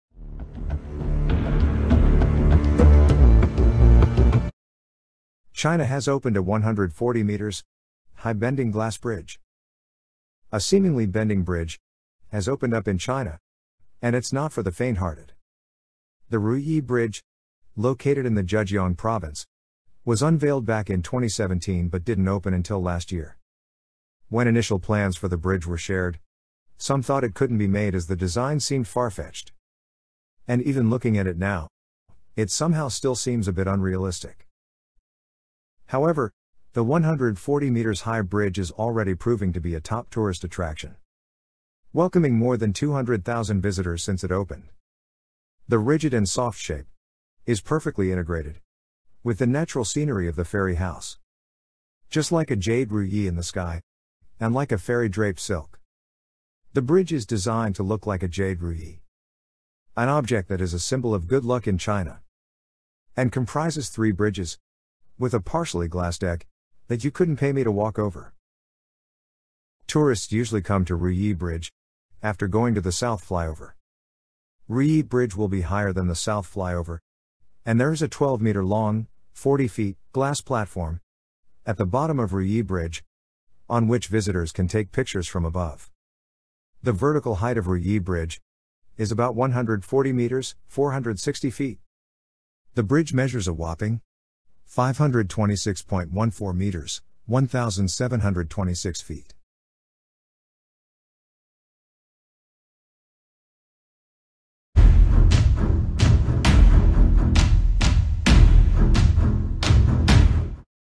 A 140m High 'Curved' Glass Bridge
China Has Opened A 140m-High curved Glass Bridge
A seemingly bending bridge has opened up in China and it's not for the faint-hearted.
The Ruyi bridge, located in the Zhejiang Province, was unveiled back in 2017 but didn't open until last year.
When initial plans for the bridge were shared, some thought it couldn't be made as the design seemed far-fetched - and even looking at it now, it somehow still seems a bit unrealistic.
However, the 140m-high bridge is already proving to be a top tourist attraction, welcoming more than 200,000 visitors since it opened.
The bridge is designed to look like a jade ruyi - an object that is a symbol of good luck in China - and comprises three bridges with a partially glass deck that you couldn't pay me to walk over.

SOURCE: LADBIBLE
IMAGE CREDIT: PA